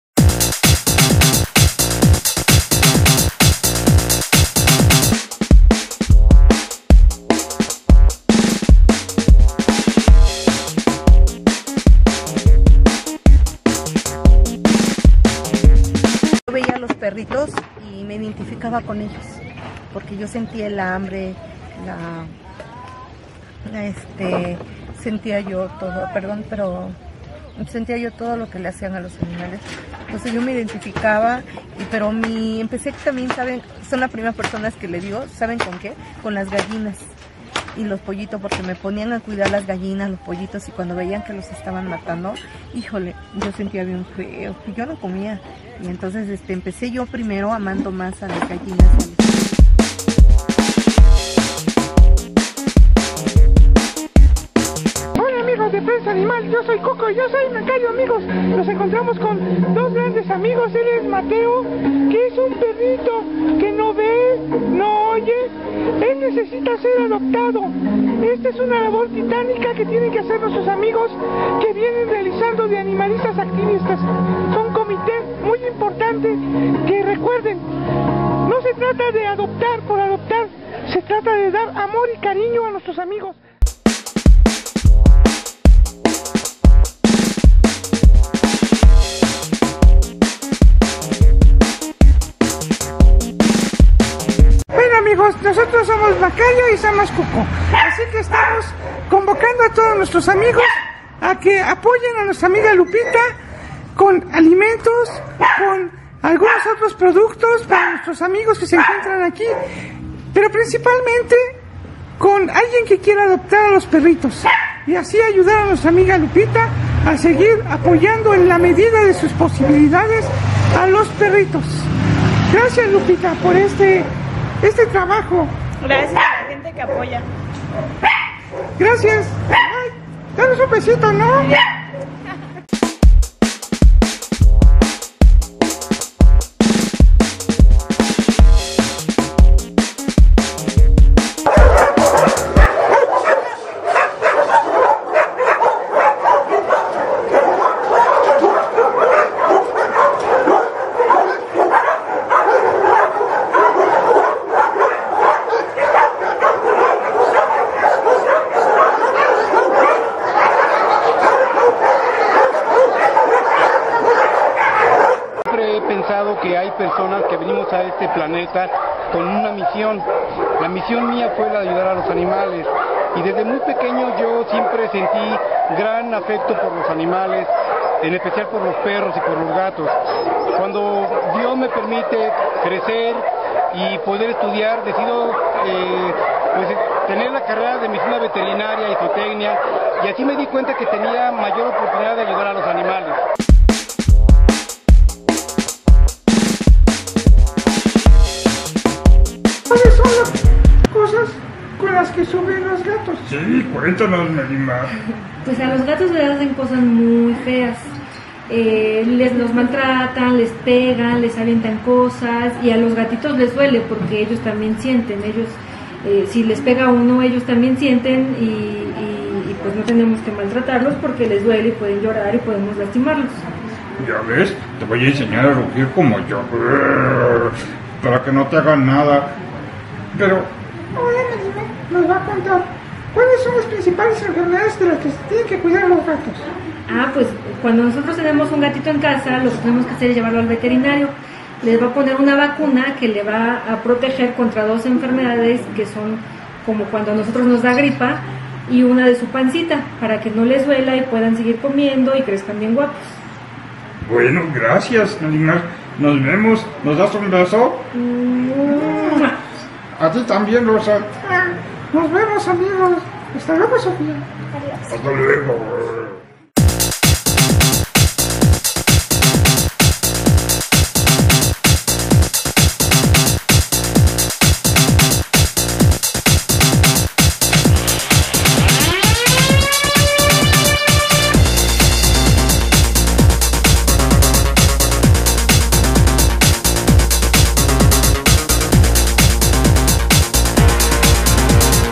La historia del 2012 en imagen
Prensa Animal hace un recuneto de algunas de las imágenes más importantes que nos ocuparon este año y nos conmovieron...